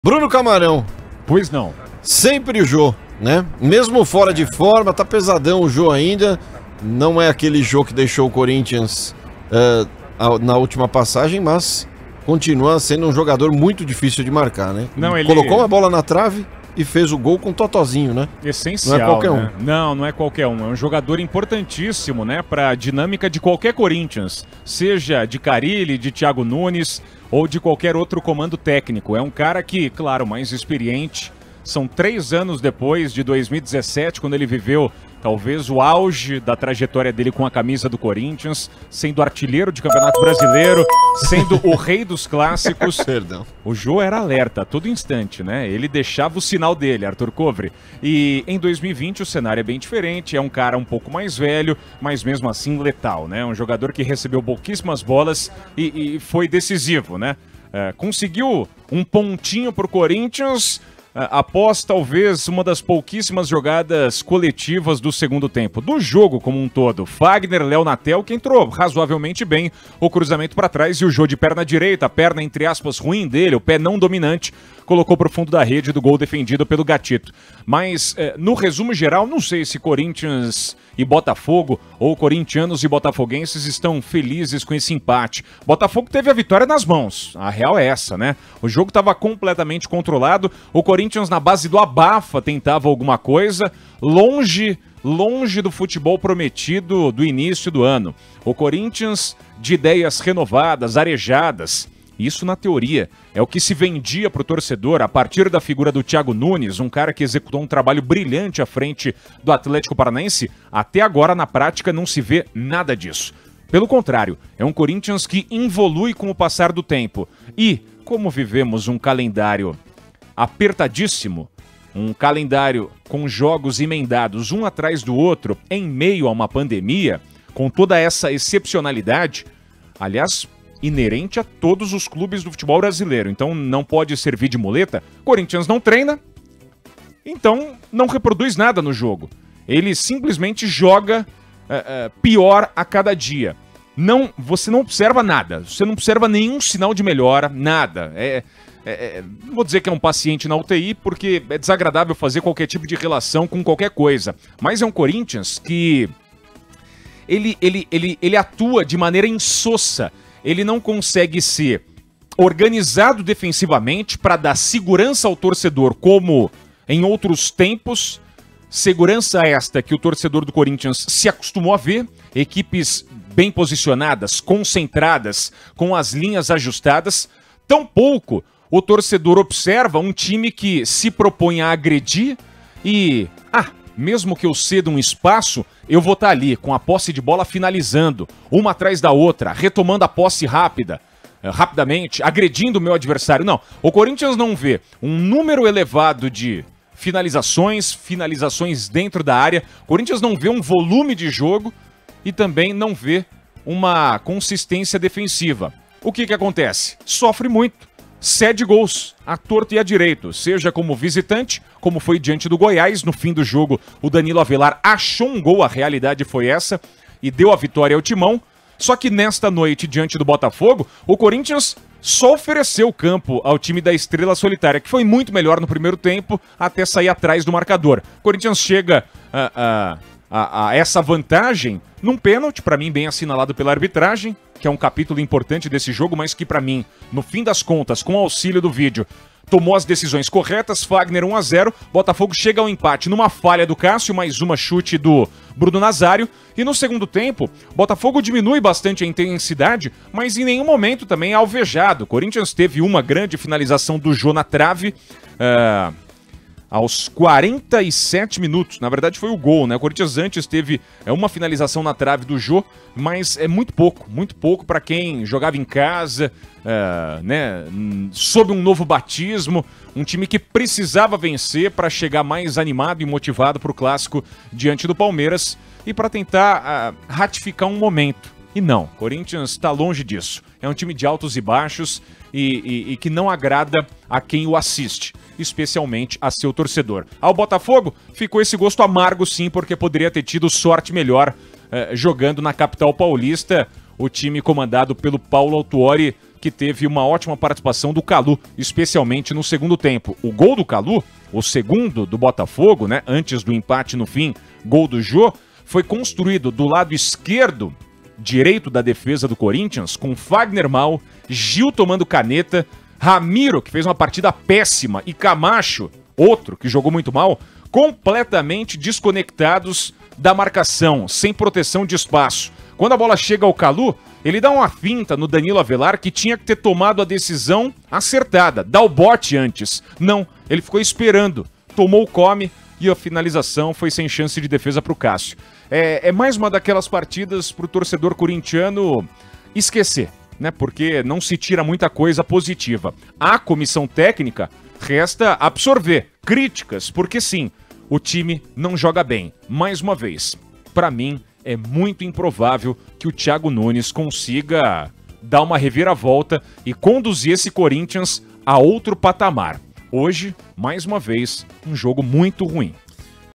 Bruno Camarão analisa empate entre Corinthians e Botafogo
Corinthians e Botafogo ficaram no empate por 2 a 2 pela oitava rodada do Campeonato Brasileiro. Bruno Camarão (@brunocamarao) reforçou a atuação de Jô durante a partida, a quem considera “importantíssimo” para todos os elencos do Timão, independente do técnico.